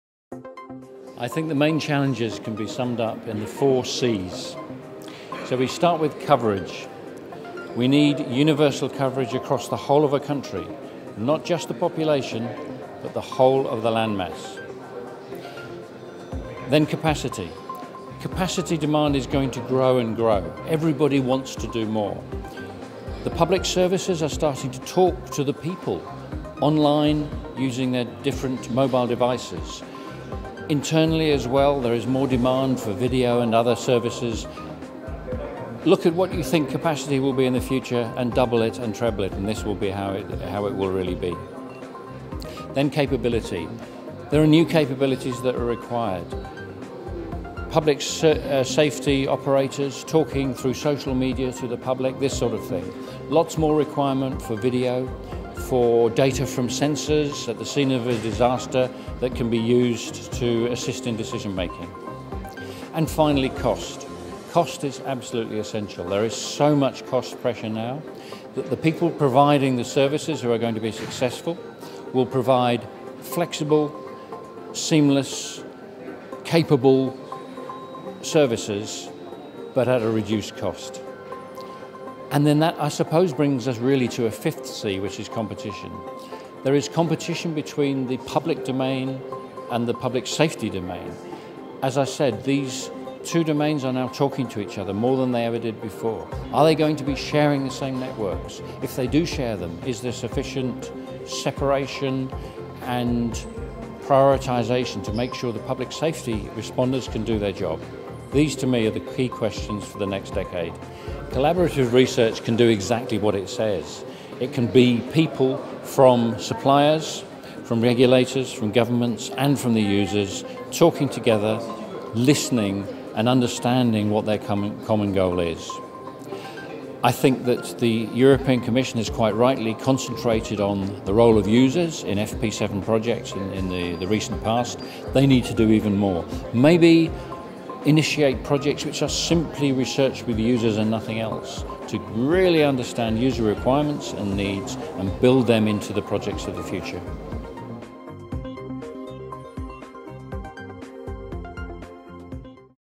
Jim Strather, British Association of Public Safety Communications Officials, UK
video blog from Future Networks Mobile Summit 2013